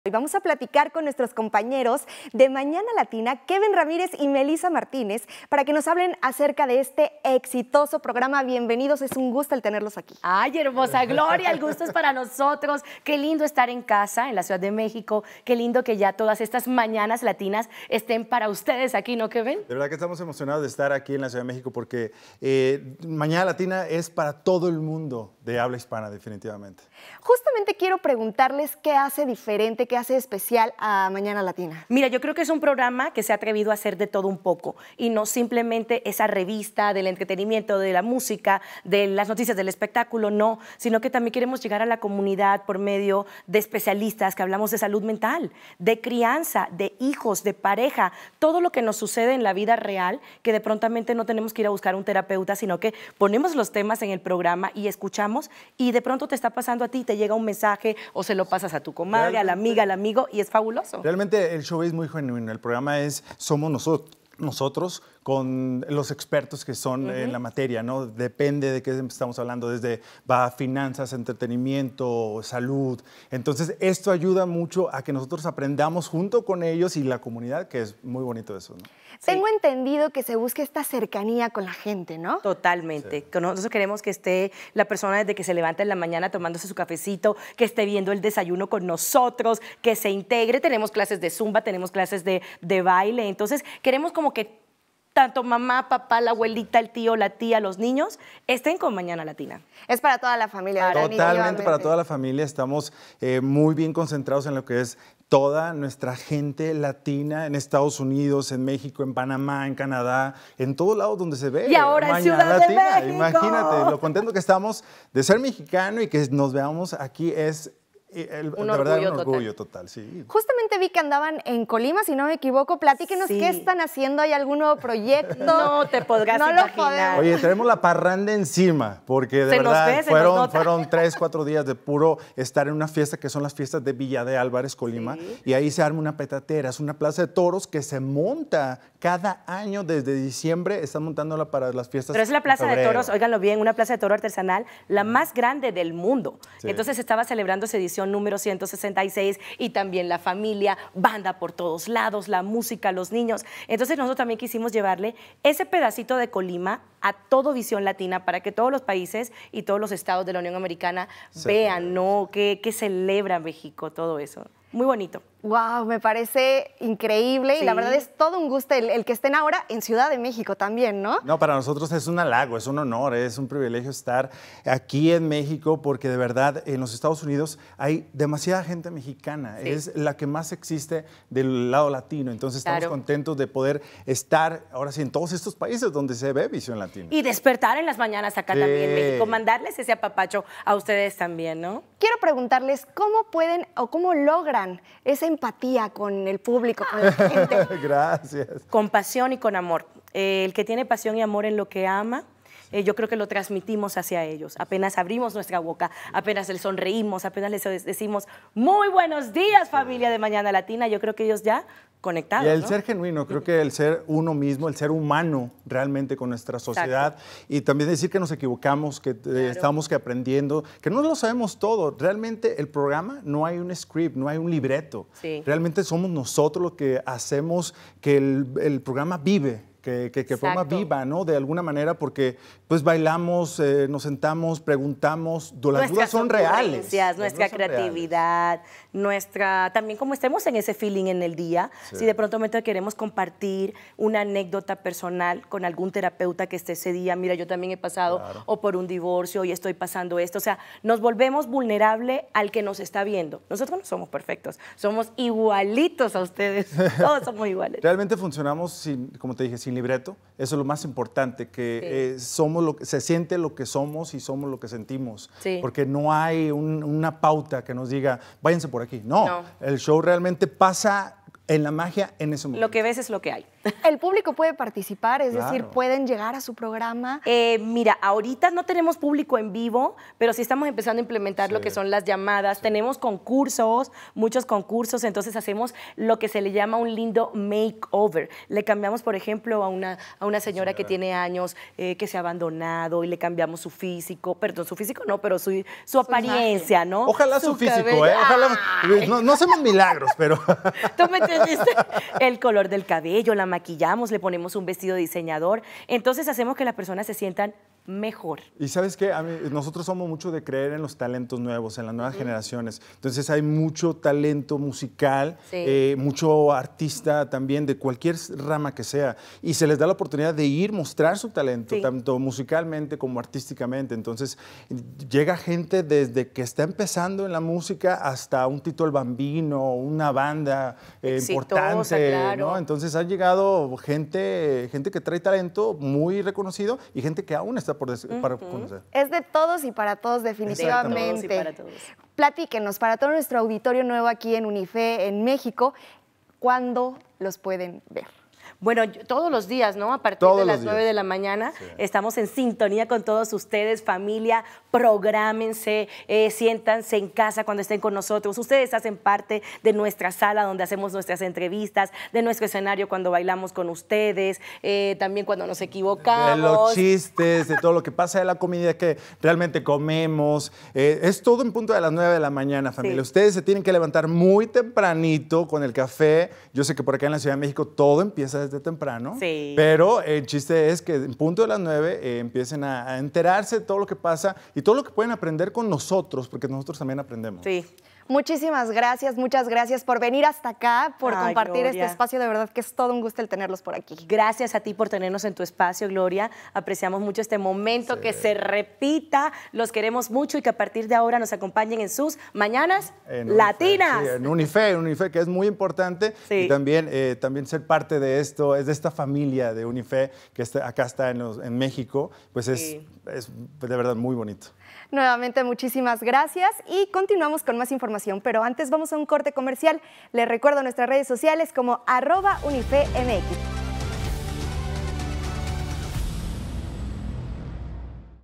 Entrevista con Melissa Martínez y Kevin Ramírez de Mañanas Latinas en Noticias 360 por UNIFE México
Gloria Guinzberg entrevista a los conductores de Mañana Latina parte de la familia